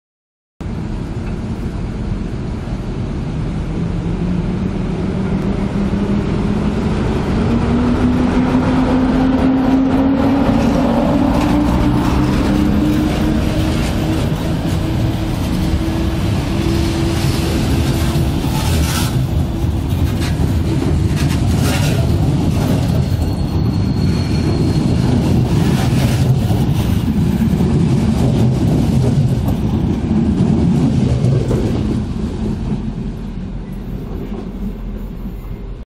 EU07-234 z EIC 81402 Błękitna Fala odjeżdża ze stacji Szczecin Główny.
EU07-234 z EIC 81402 Błękitna Fala relacji: Świnoujście - Warszawa Wschodnia odjeżdża ze stacji Szczecin Główny.